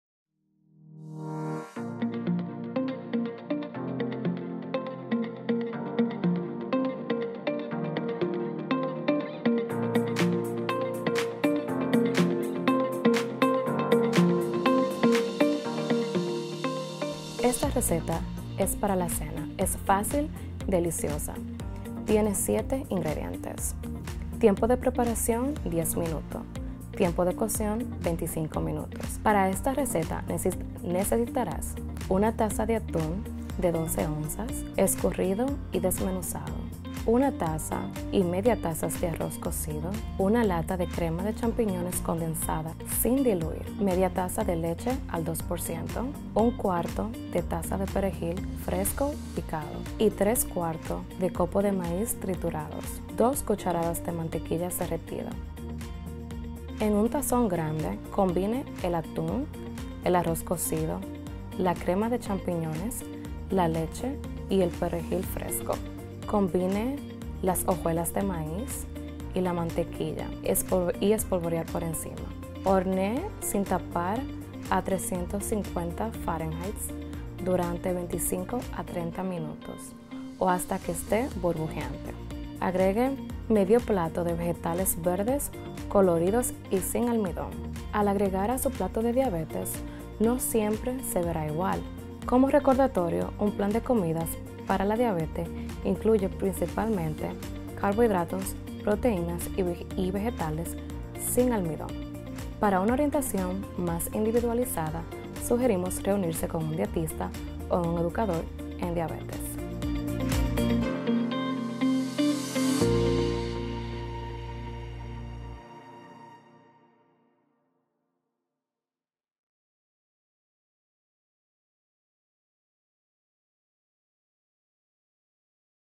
Atún Crujiente Sorpresa
El atún crujiente sorpresa es una receta amigable para quienes llevan un estilo de vida con diabetes.